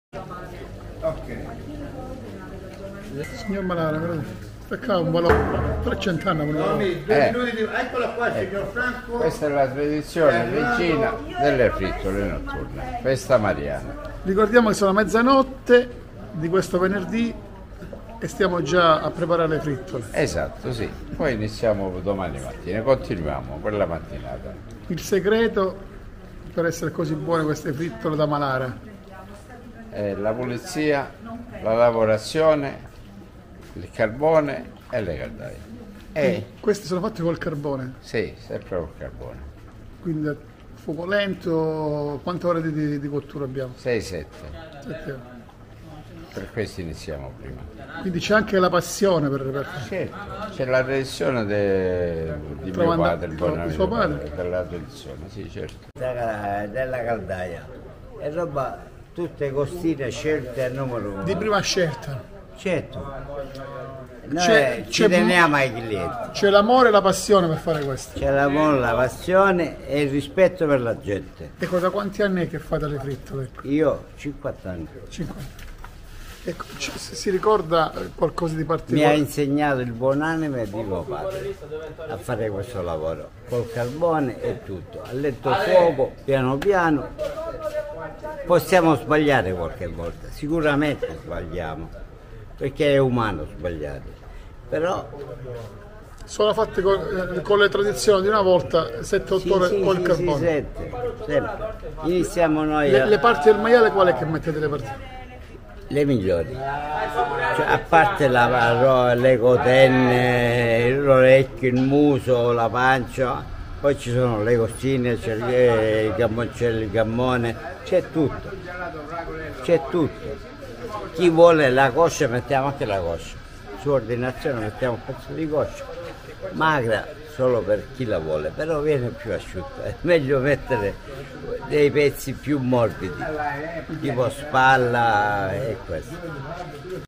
Reggio Calabria: Festeggiamenti per la Madonna, il rito delle frittole
Le frittole sono pezzi di maiale cotti in un pentolone d’acciaio chiamato caddara e che si preparano durante i mesi più freddi. Si cuociono riempiendo la caddara di grasso, poi, quando questo si scioglie diventando strutto, si mettono nel pentolone anche altre parti del maiale (lingua, orecchie, muso, pancia, gamboni, cotenne, costolette e salsicce). La cottura in media dura 8 ore e durante tutto questo tempo la caddara, posta davanti a una macelleria, incomincia a spandere i suoi aromi.